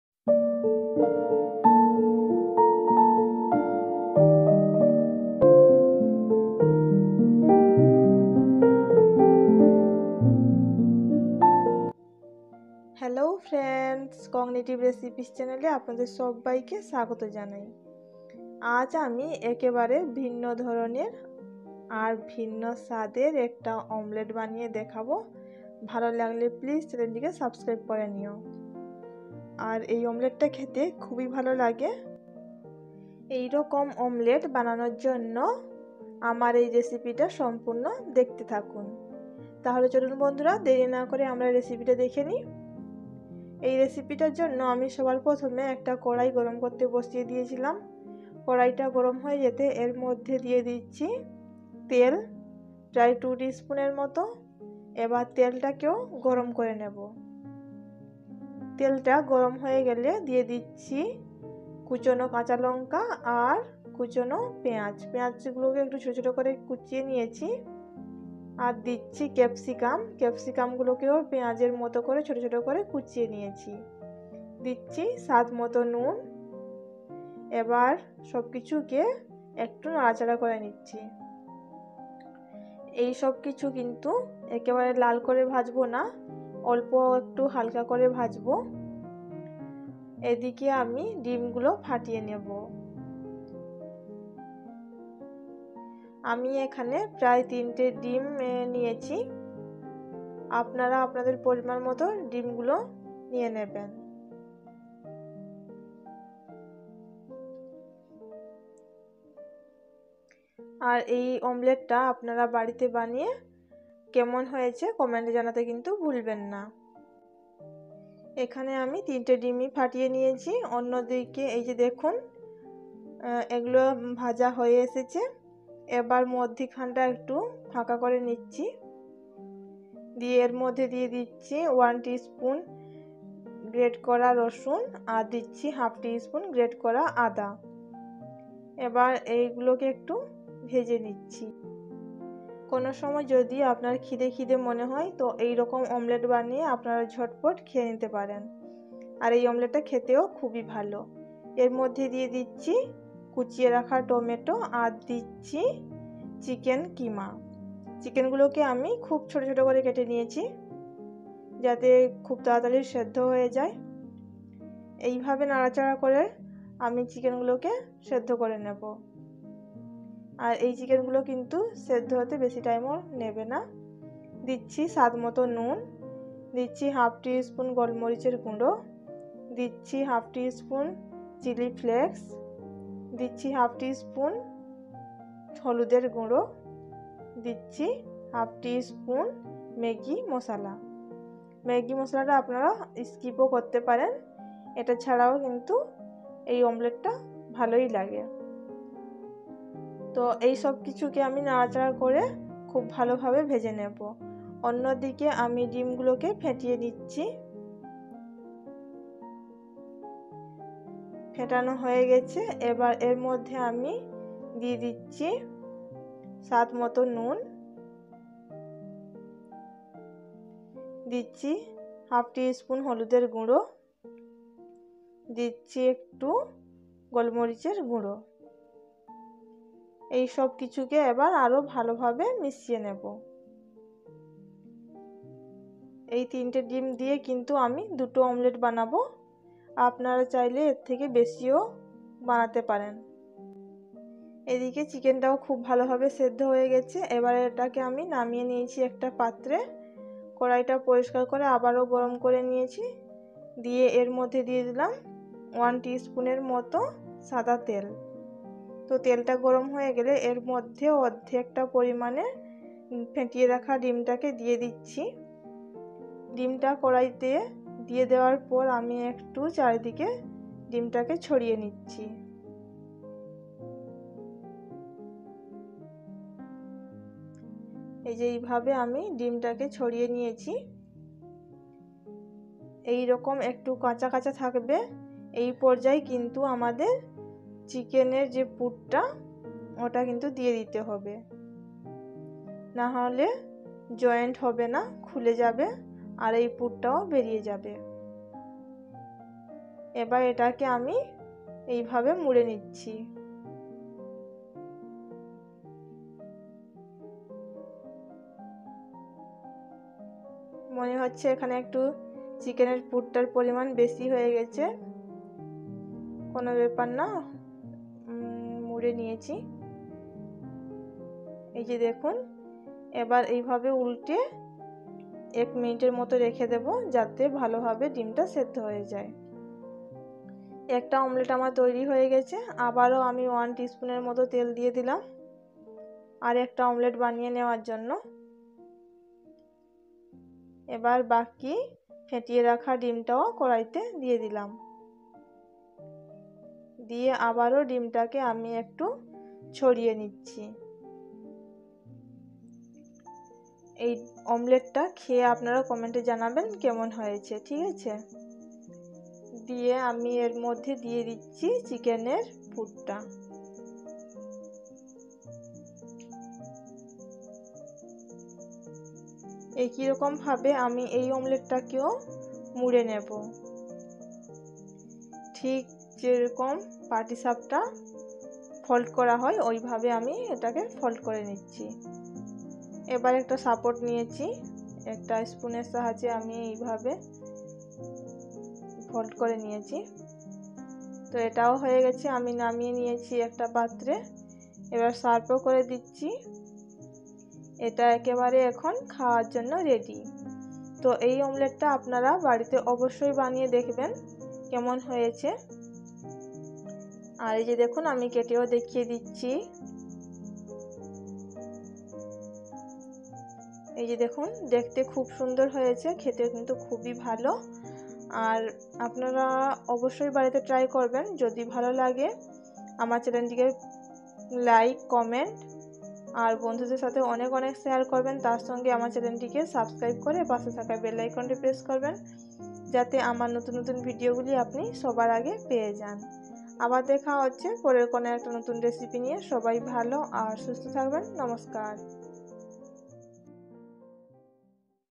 Yammy Egg Omelette Recipe ! অল্প সময়ে তৈরি করুন স্পেশাল ডিমের অমলেট রেসিপি ! @Cognitive Recipes
Ingredients:-

1. egg(3 pice)
2. oil
3. salt
4. black pepper powder
5. chilli flakes
6. green chilli
7. onion
8. capcicum
9. chicken kima
10. turmeric powder
11. garlic
12. ginger
13. Maggie masala